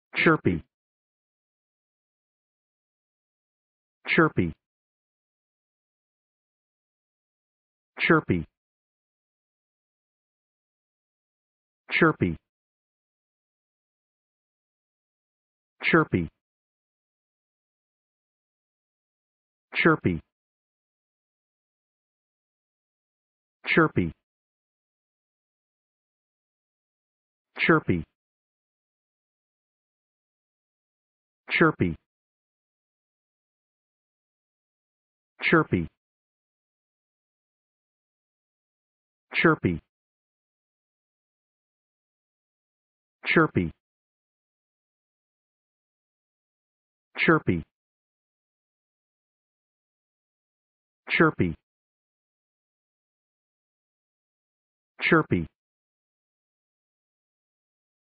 Pronunciation of Chirpy | Definition of Chirpy
Chirpy pronunciation | How to pronounce Chirpy in English?
/`tʃɜːrpiː/
Meaning of Chirpy | What is Chirpy?
(1) (adjective) characterized by liveliness and lightheartedness; (2) (birds or insects) characterized by or tending to chirp;
What is Chirpy | Chirpy meaning and definition?